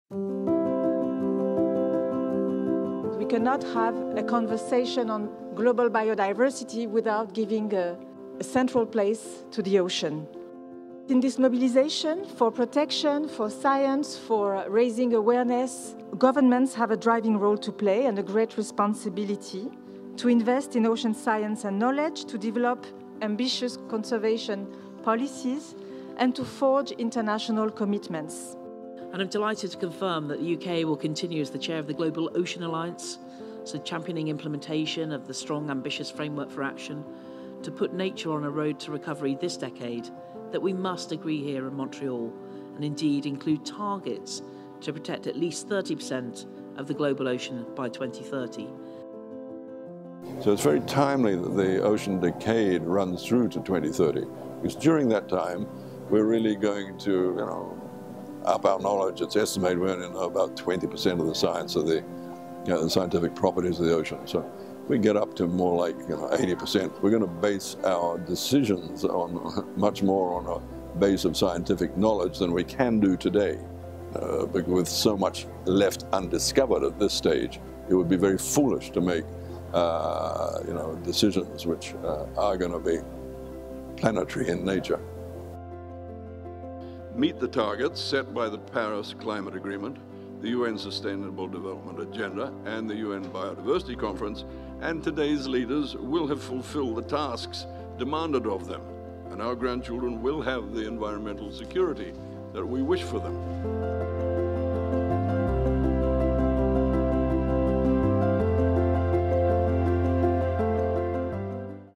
An Ocean of Life - the flagship Ocean event at COP15
The post-2020 Global Biodiversity Framework, the most ambitious global plan ever developed for biodiversity, was the central focus of negotiations at the 15th Convention of Parties of the Convention of Biological Diversity in Montreal, Canada, 5 – 17 December 2023. A major contribution to the achievement of the 2030 Agenda, the Framework aspires to create a world living in harmony with nature and has an increased focus on marine and coastal biodiversity. Successful implementation of the Framework will require increased investment in the generation and uptake of relevant ocean science and knowledge, and partnerships across diverse groups of actors to transform science and knowledge to scalable action and practical solutions.

The Intergovernmental Oceanographic Commission of UNESCO together with the UN Decade of Ocean Science for Sustainable Development, was delighted to host a flagship ocean event at COP15: "An Ocean of Life: Knowledge and Solutions for Marine and Coastal Biodiversity under the Post-2020 Global Biodiversity Framework"